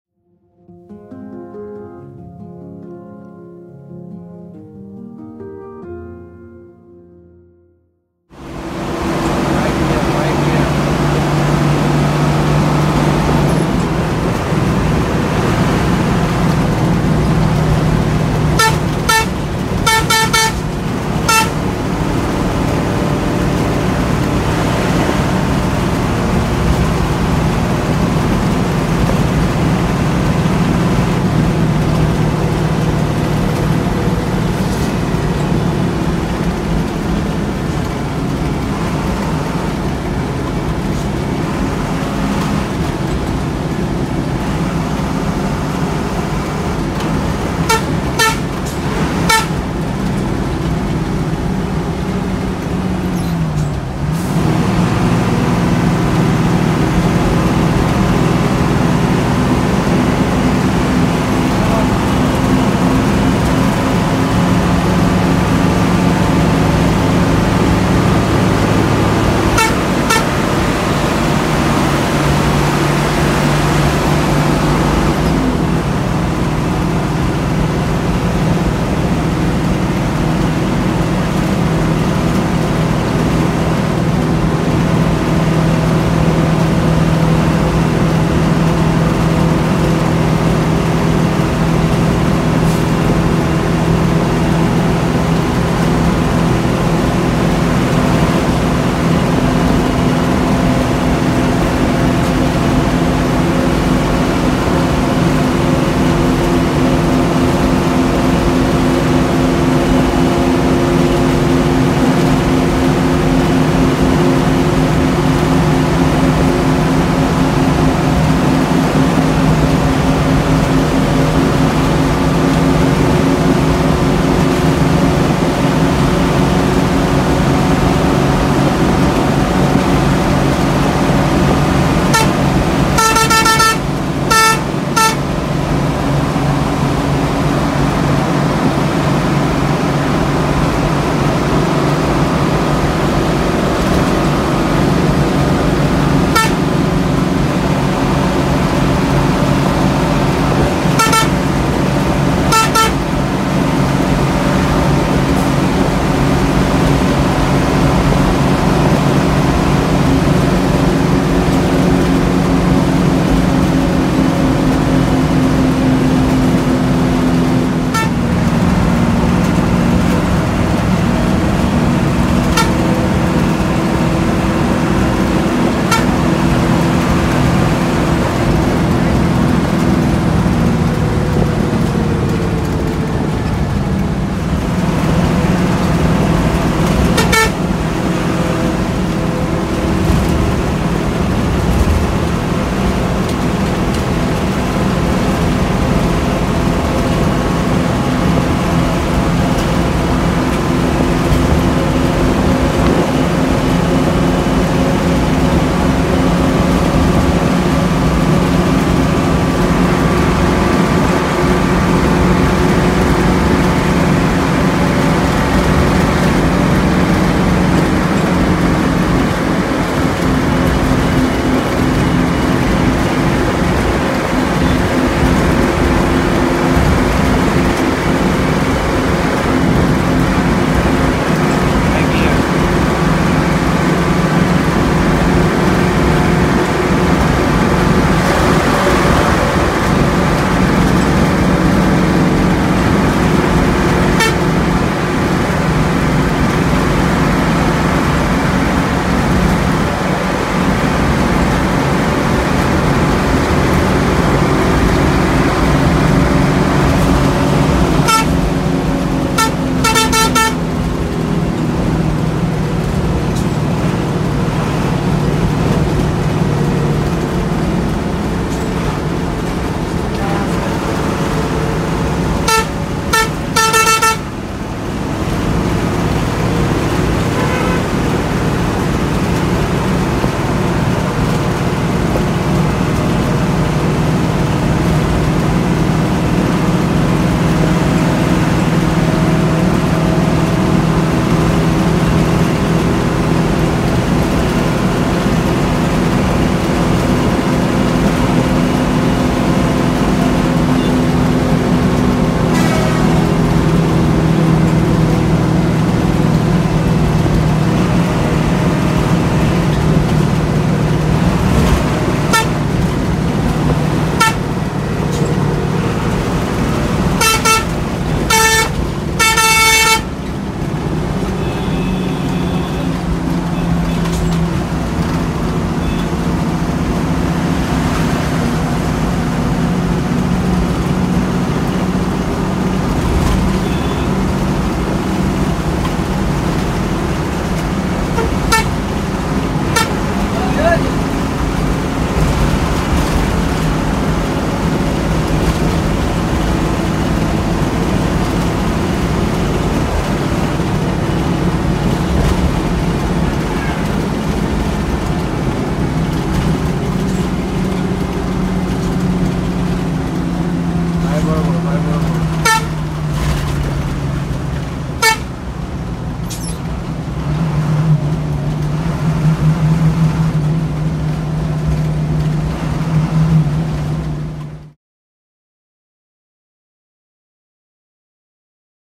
স্টার লাইন বাস 2532 এর গতি 🔥॥ Jahangir Bhai ॥ S3 Episode 21
This Video has done by a Professional.
Please Do not try this On Road.

Operator: Star Line
Driver: Jahangir Bhai
Registration: 2532
Route: Dhaka - Chittagong Highway